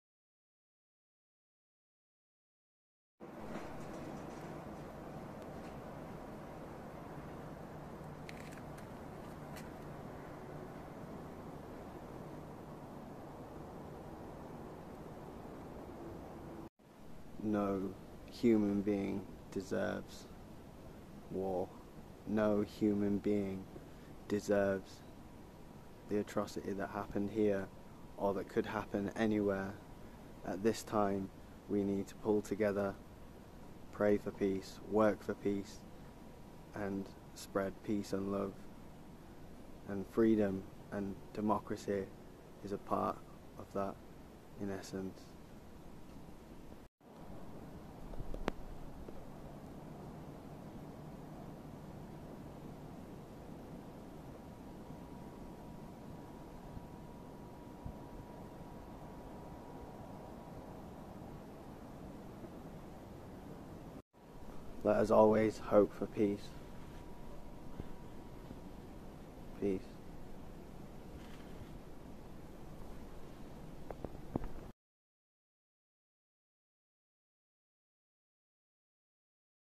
Yorkshire Man in Japan Never forget Nagasaki
We cannot forget the devastation that nuclear weapons can unleash. I do not speak for the Japanese I speak as a member of the human family. It has taken great courage to rebuild Nagasaki.
Leaders never lay the bricks to build a city but they will so easily order the bombs to bring them down. I cannot imagine what is going through the minds of the people in the city below as through this day they have had to learn of Putin's vile threat.